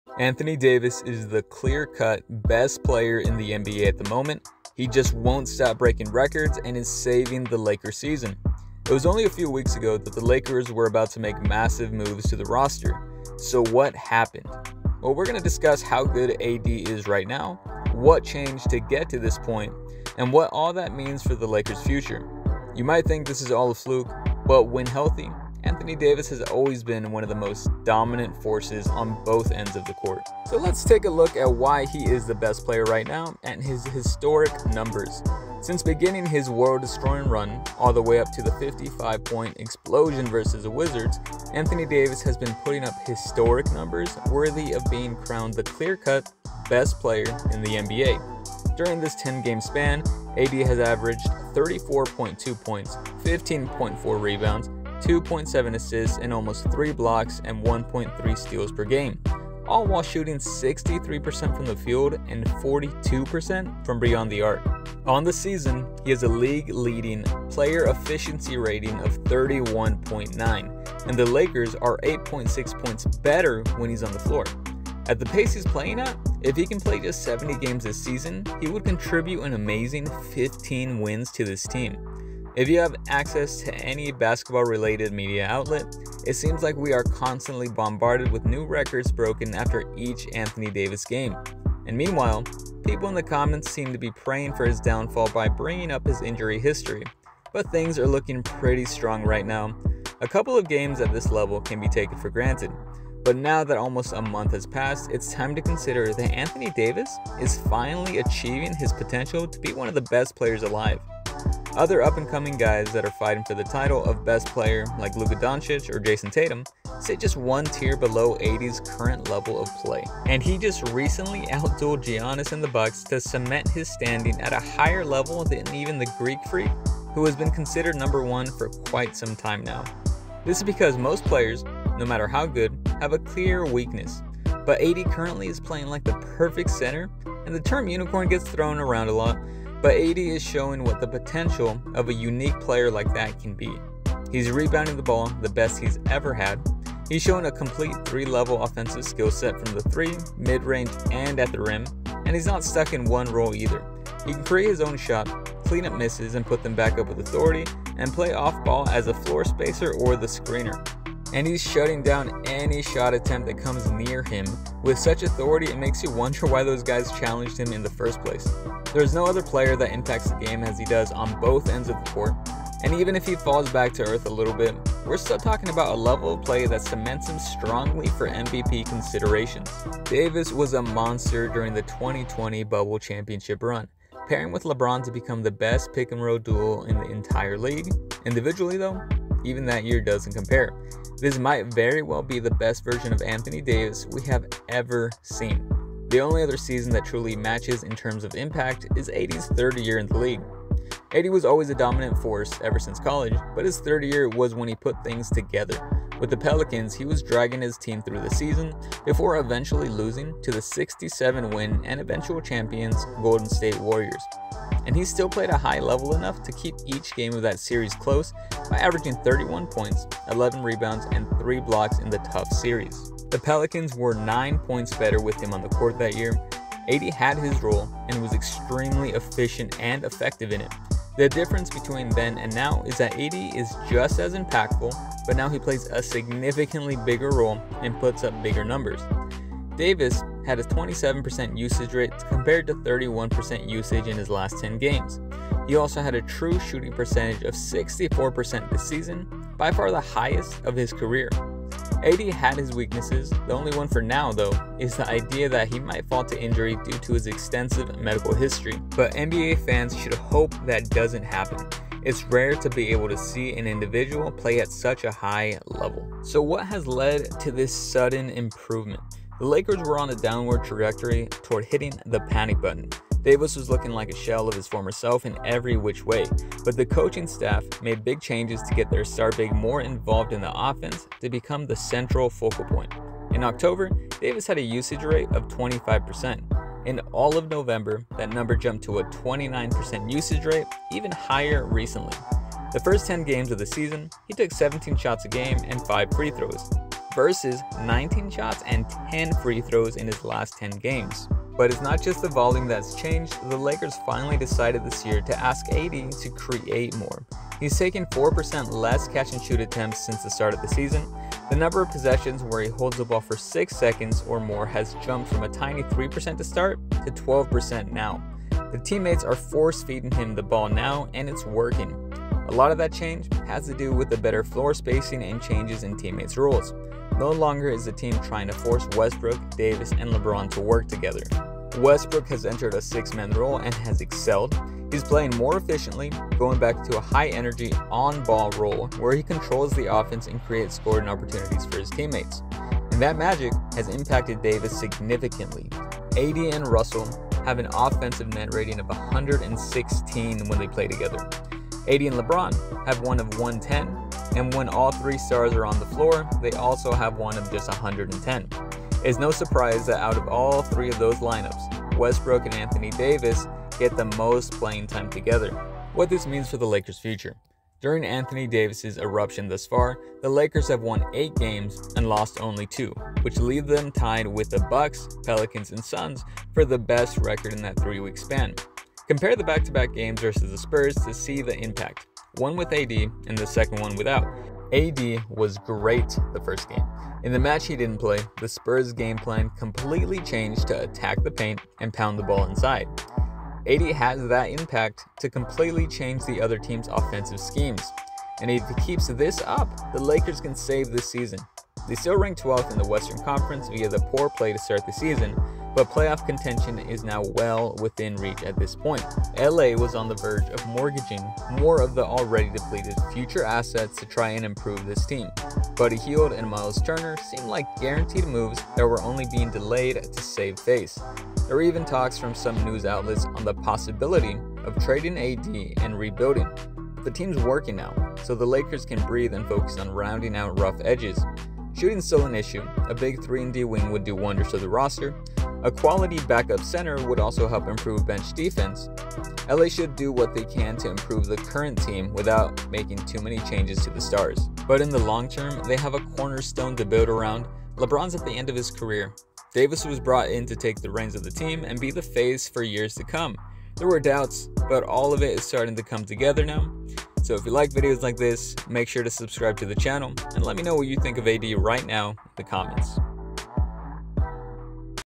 Anthony Davis Has Evolved To A New Level
Anthony Davis has been unstoppable the past month. He is carrying the Lakers through the schedule and gathering historic numbers along the way. Look out NBA teams, there is a new best player climbing to the top of the throne.